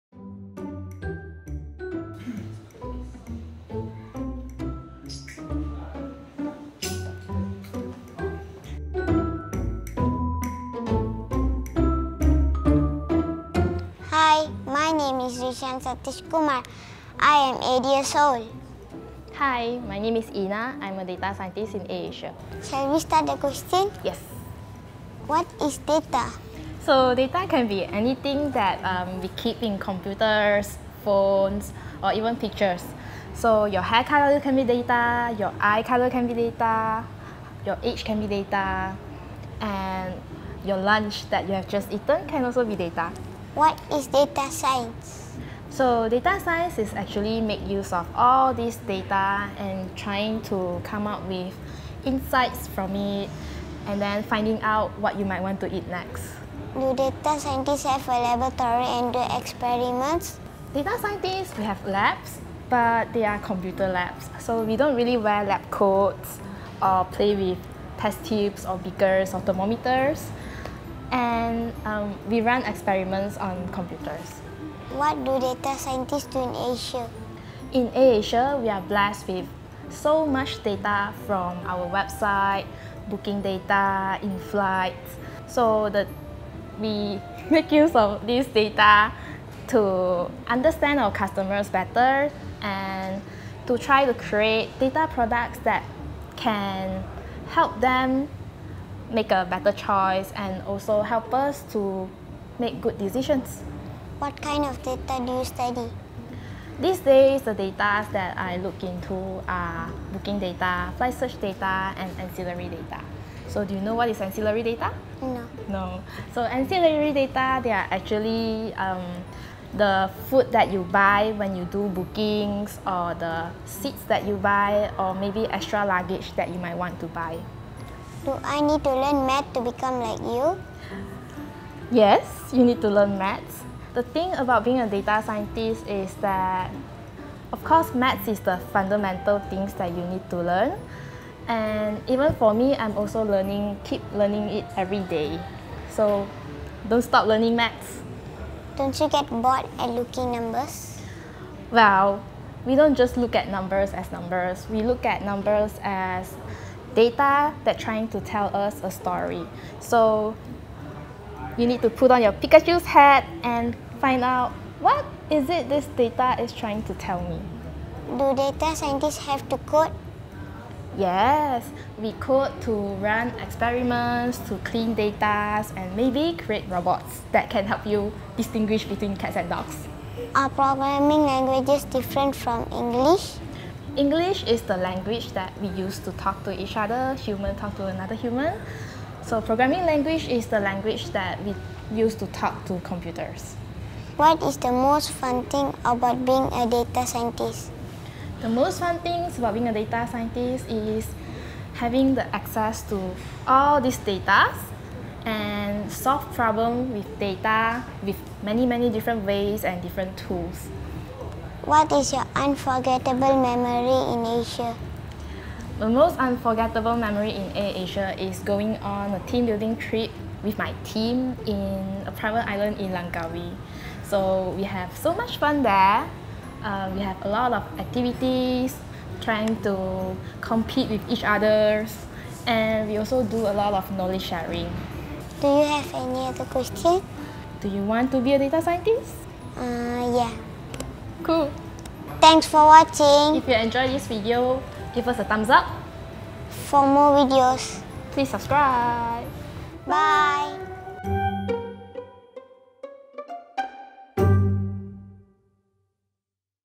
AirAsia | AskAnAllstar: Kid Interviews A Data Scientist
Rishan discovers the basics of data science with our Data Scientist, Teoh Ee Na in the final episode of AskAnAllstar